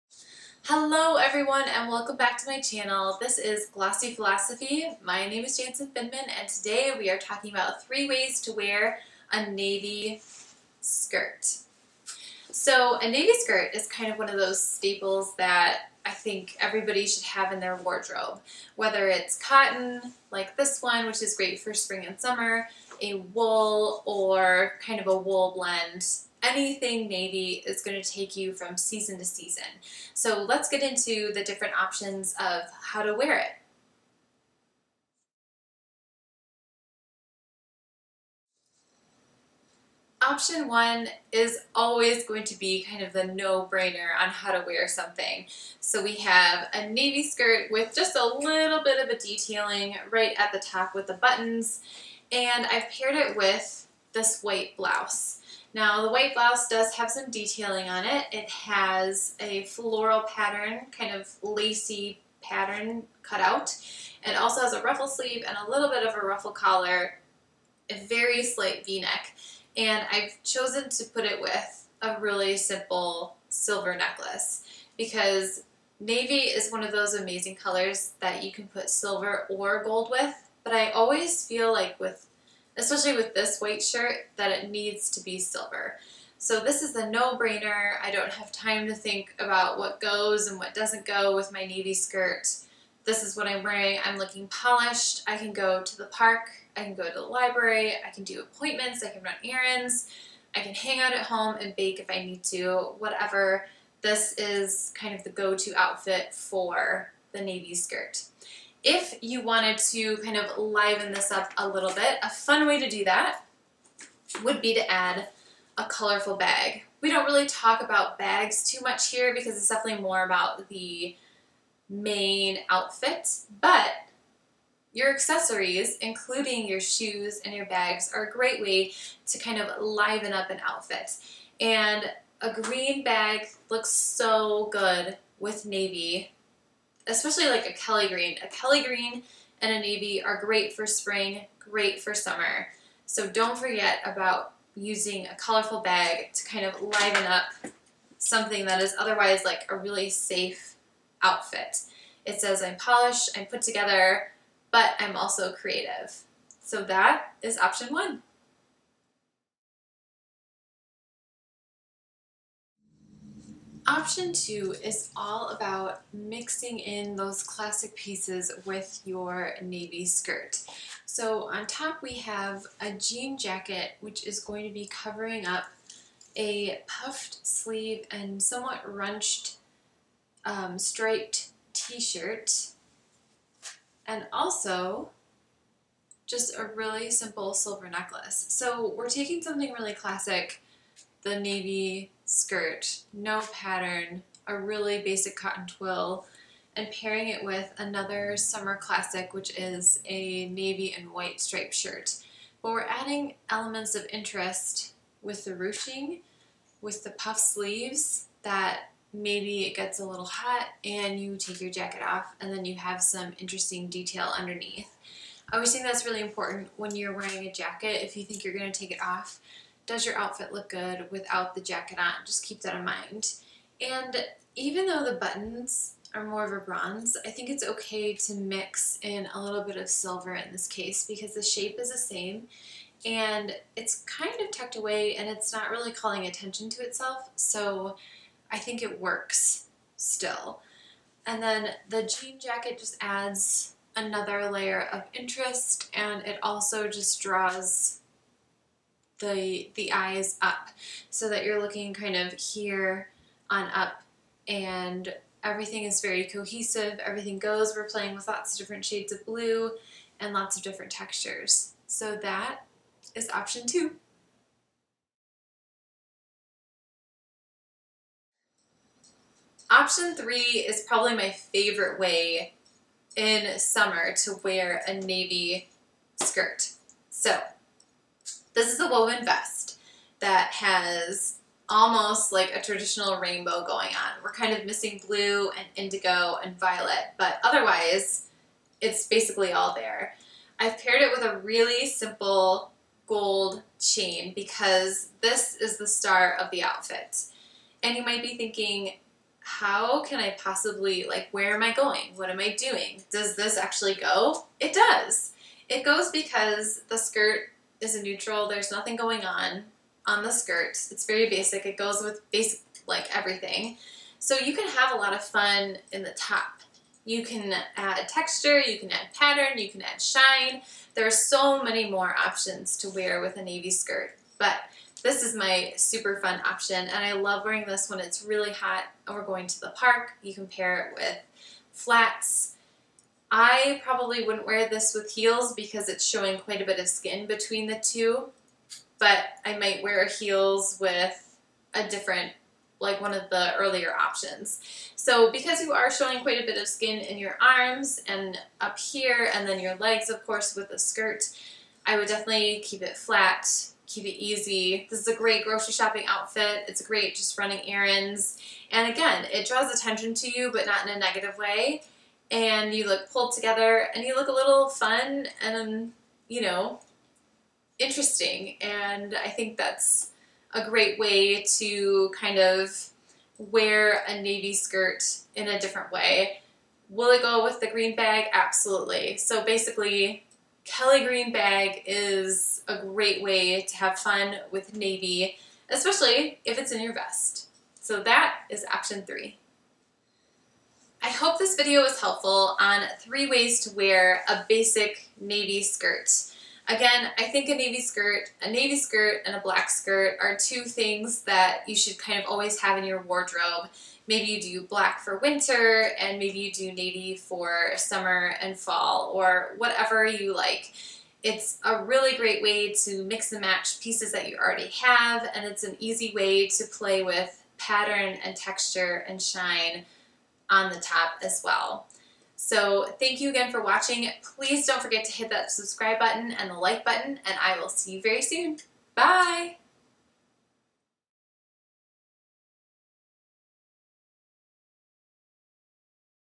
3 Ways to Wear a Navy Skirt | Summer Fashion
Hello Everyone! Today we are talking about 3 Ways to Wear a Navy Skirt. A Navy Skirt is a staple for any woman, mini, midi, maxi, etc. This versatile piece of clothing will take you from the park to the office. Enjoy how I style my anntaylorloft skirt 3 different ways for the summer.

CLOTHING MENTIONED:
OPTION 1:
-Skirt: Admiral Pocket Skirt - Ann Taylor LOFT
-Top: Eyelet Ruffle Split Neck Shell - Ann Taylor LOFT

OPTION 2:
-Jacket: Classic Jean Jacket | J Crew
-Top: Striped Crinkle Puff Sleeve Top | Ann Taylor LOFT
-Shoes: (same as option 1)

OPTION 3:
-Necklace: Kendra Scott | Camry Layered Pendant Necklace